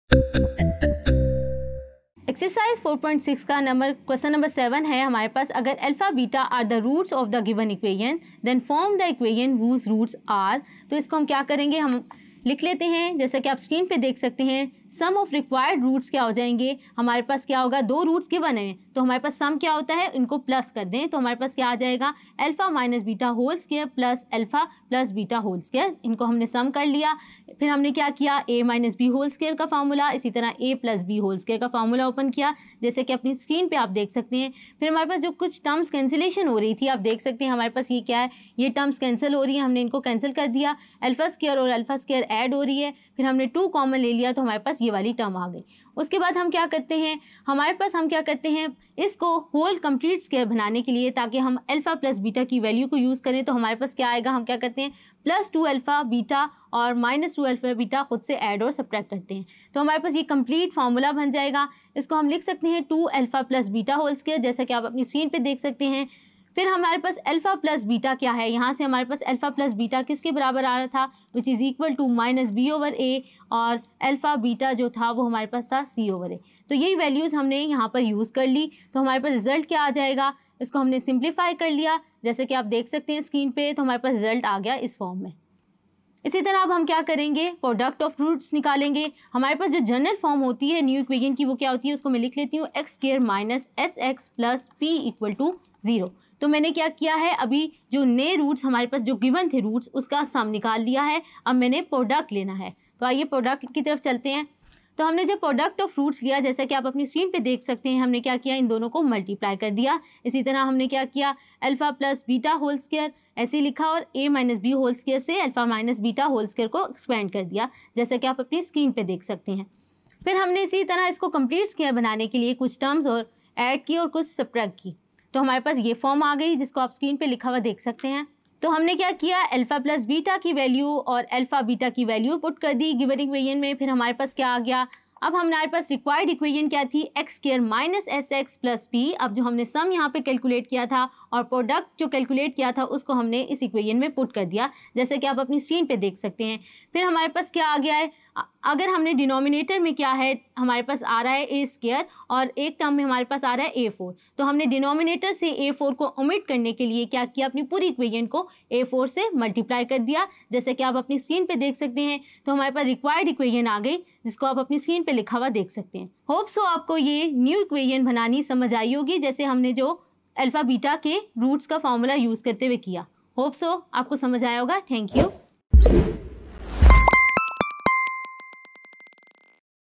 Maths Class 11 | Chapter 4 | Topic 33 | Exercise 4.6 | Question 7 | Part-vii | in urdu | tutoria.pk
Let’s take a look at the Chapter 4 of Mathematics of Grade 11 “Quadratic Equations”.

In this chapter the teacher has given a detailed explanation about the Quadratic Equations. Other topics like Solution of Quadratic Equation, Exponential Equations, Nature of the roots of Quadratic Equation, System of Equations in Two Variables, and Word Problems on Quadratic Equations have been covered in this video.

In this video, you will study Mathematics Grade 11 Chapter 4 Topic 33 "Exercise 4.6 | Question 7" by instructor Samra Maqsood.